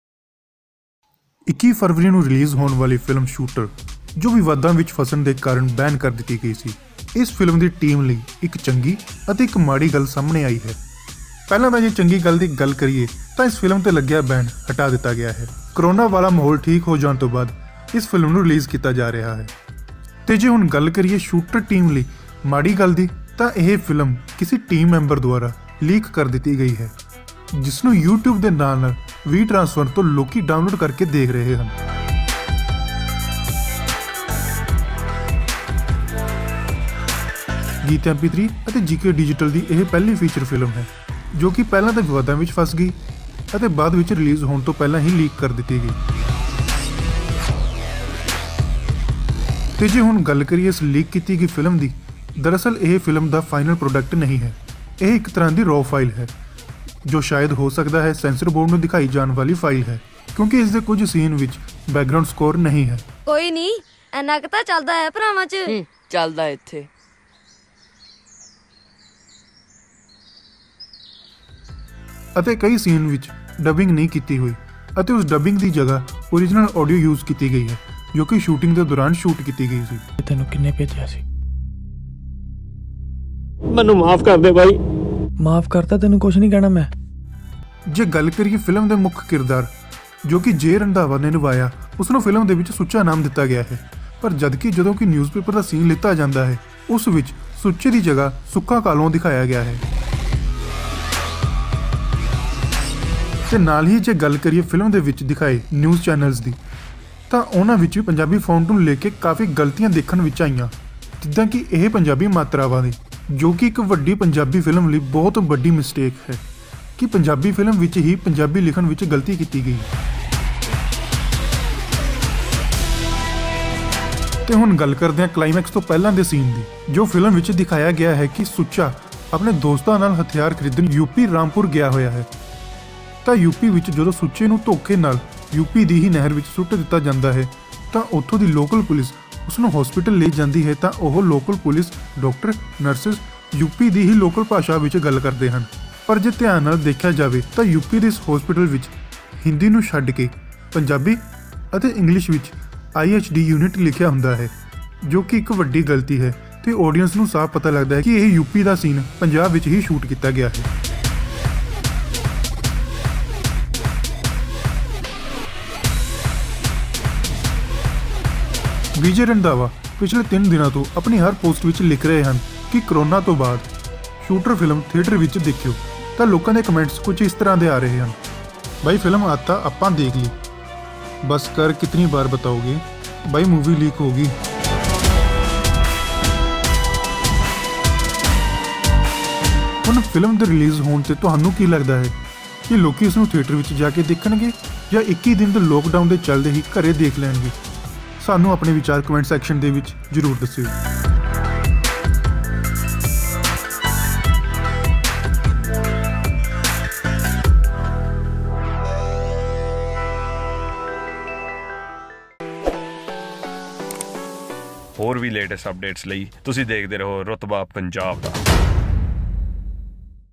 5 Big Mistakes In Shooter Movie | Pollywood Updates | Rutbahh Punjab Da
shooter film diya 5 badi galtiya, te naal hi dekho eh film kis ne leak Kiti

If you want to suggest us to try something new...SUGGESTIONS ARE WELCOME.

FEEL FREE TO WRITE YOUR REACTIONS IN COMMENT SECTION..!!!